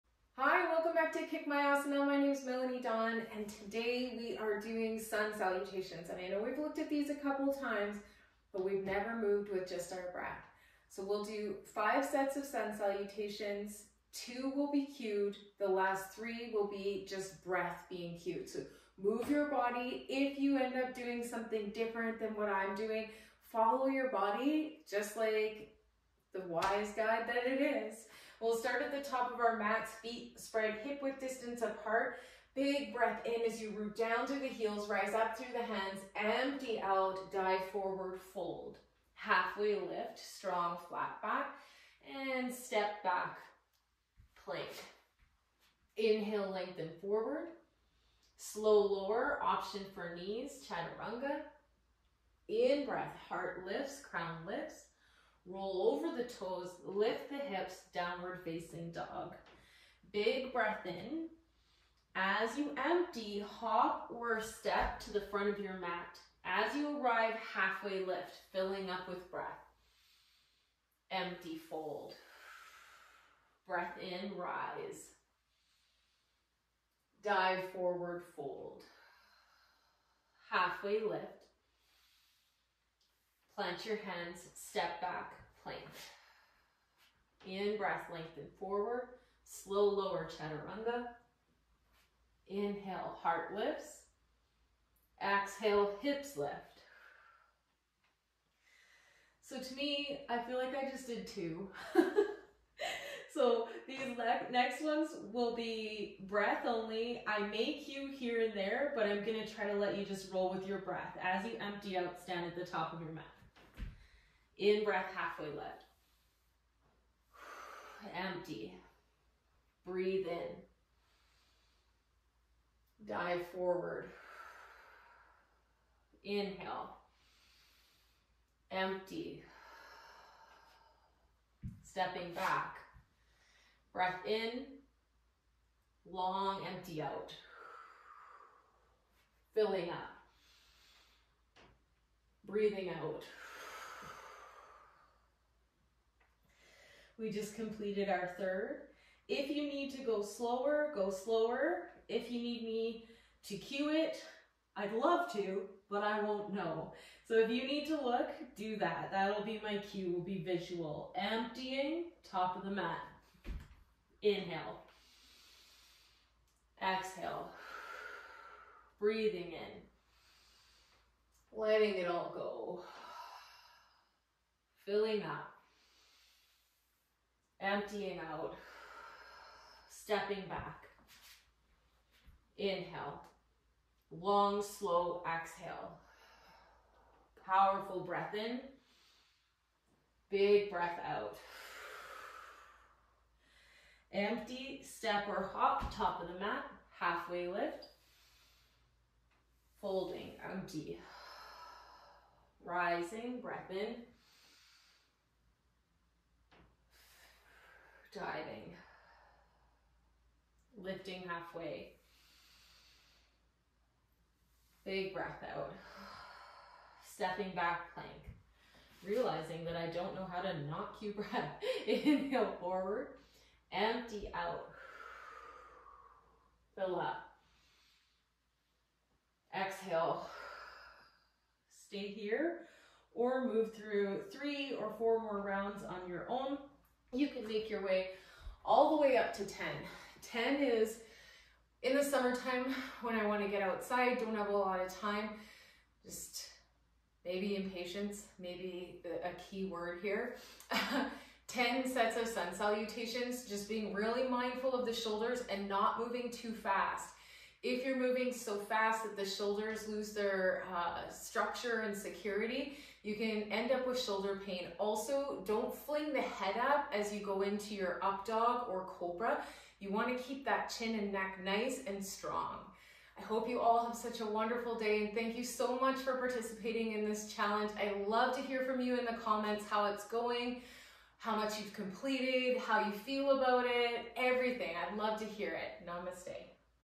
Kick My Asana Yoga Challenge - Day 20 - Sun Salutations with Breath
I try my darndest to not cue the sequence and give you the opportunity to link up with your breath and move your body the way it wants to be moved but, alas, I am operating from cellular memory and sometimes that fails me!

Let me know what you think about this! If you ever get the chance to do this in a room full of yogis breathing in sync, it’s something you never forget.